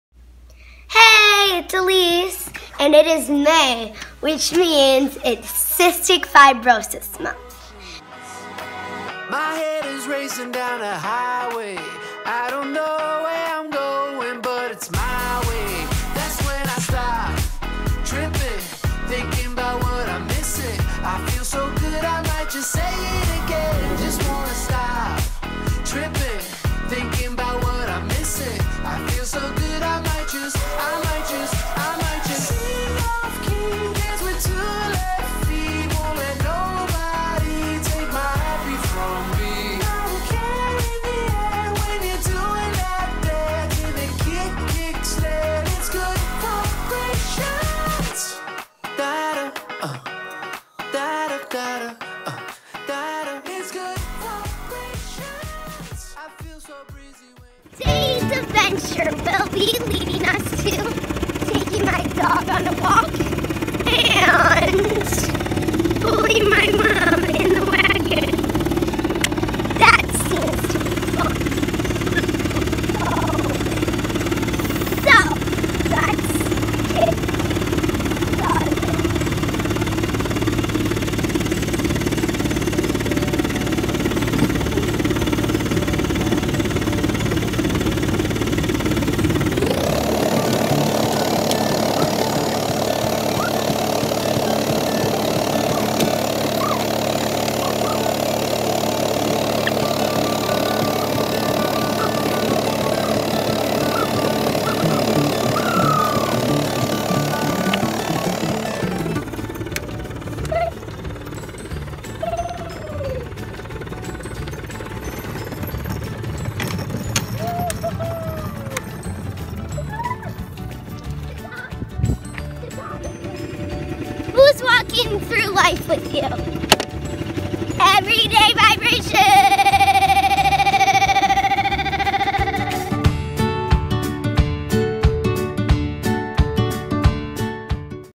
Day 10 Walking Through Life
The journey today is with Elyse and her Vest as they walk through Life...she has a heavy load she is pulling...who are you sharing life with? Who is helping you pull your burdens? Keep pressing on and pushing through!
All music rights to Reed Good Vibrations